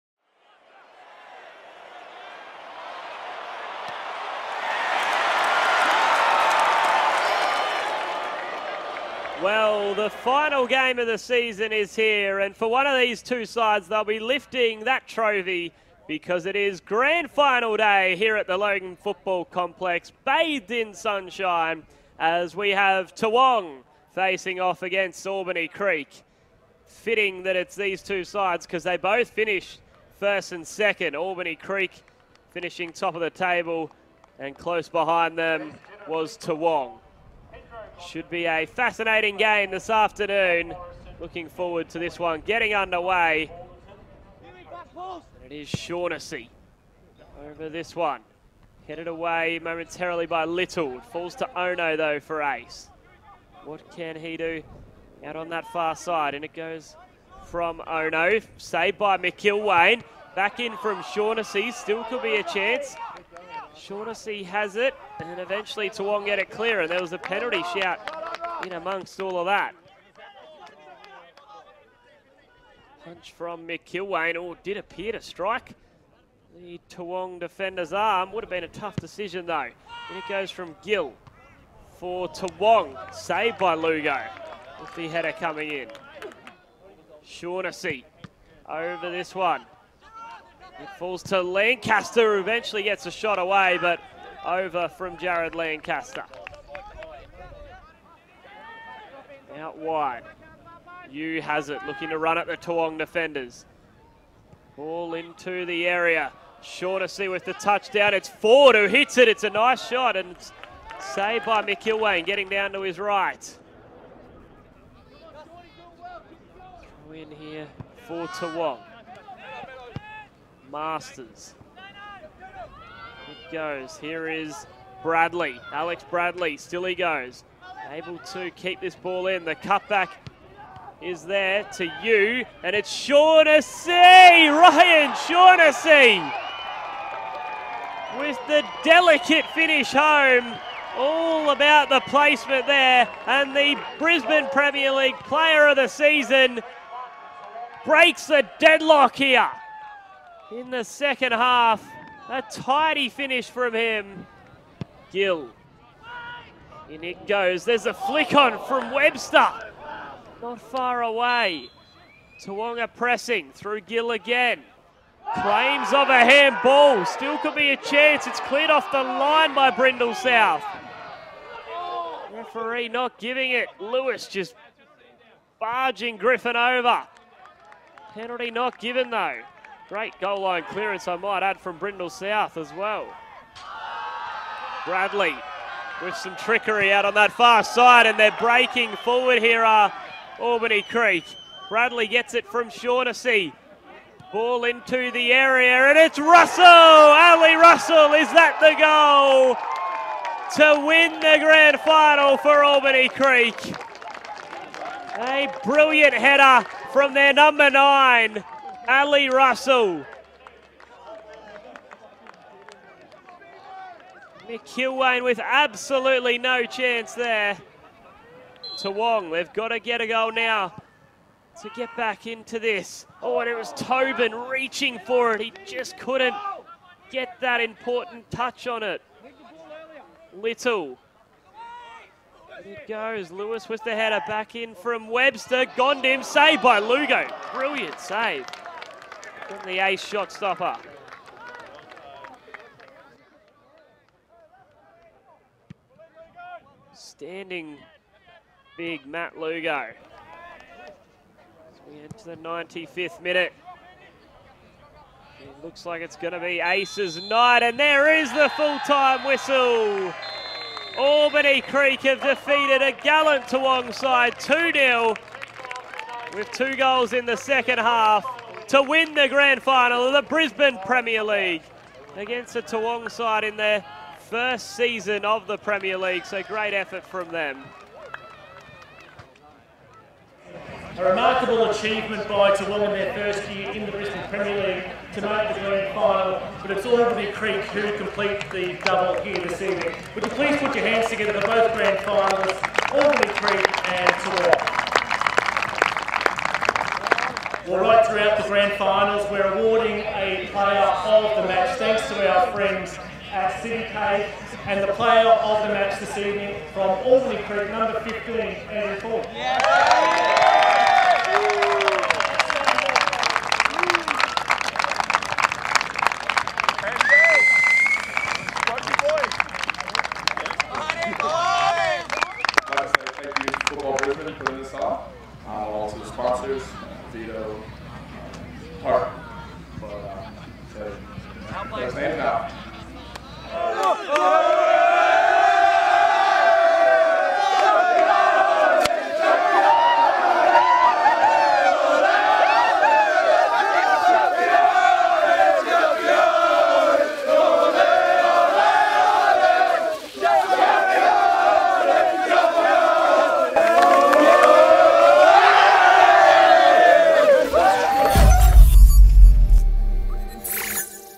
BPL GF Toowong FC v Albany Creek Excelsior
Grand Final of the Brisbane Premier League as Toowong FC who are in their first year of the league take on the 2019 Champions Albany Creek Excelsior.